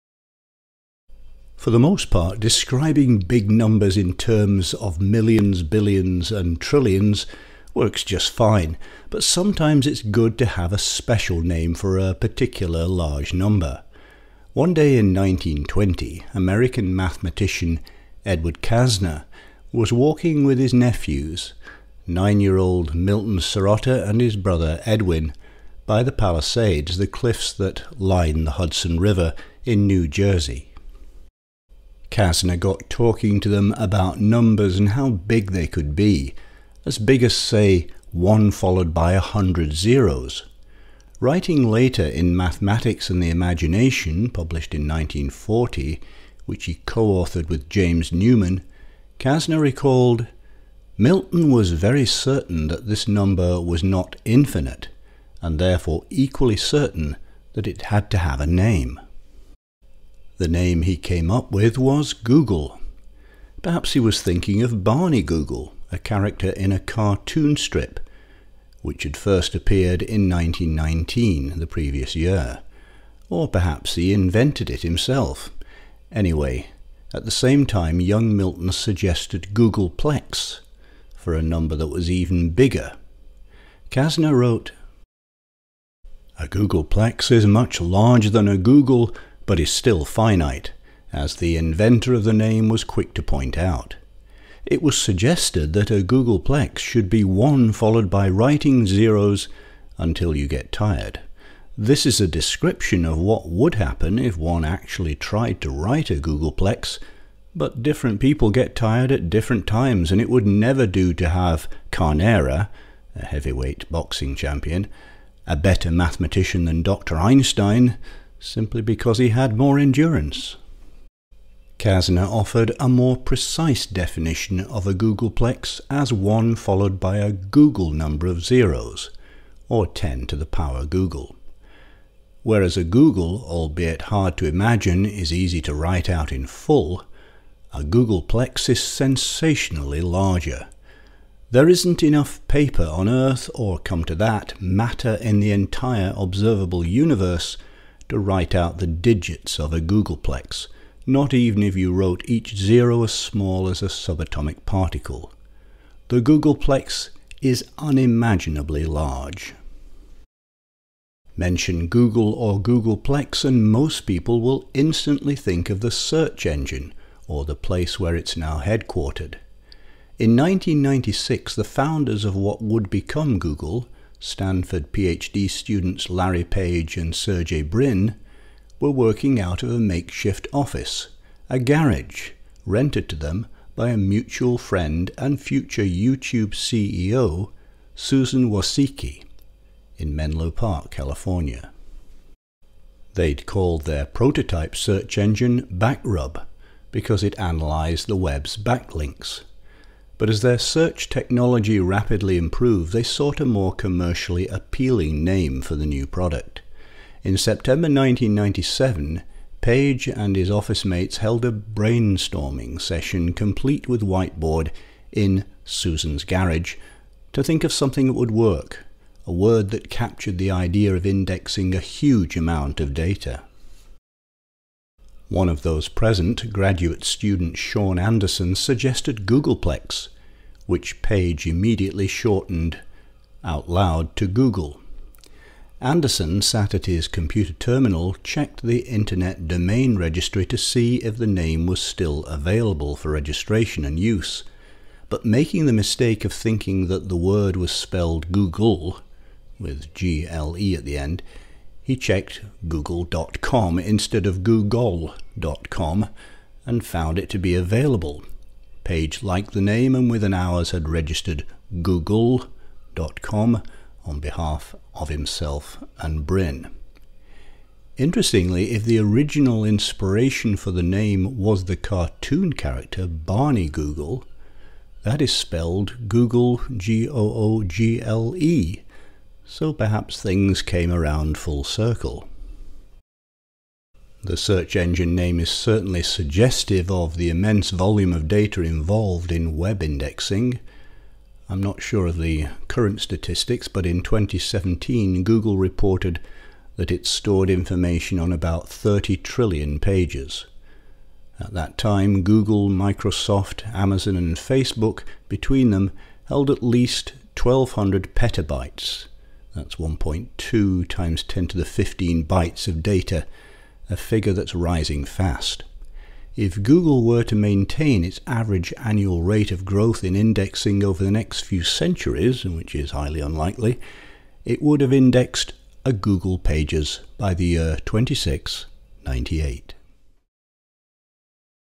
The story of the googol (and Google!)
For the most part, describing big numbers in terms of millions, billions, or trillions works just fine. But sometimes it’s good to have a special name for a particular large number.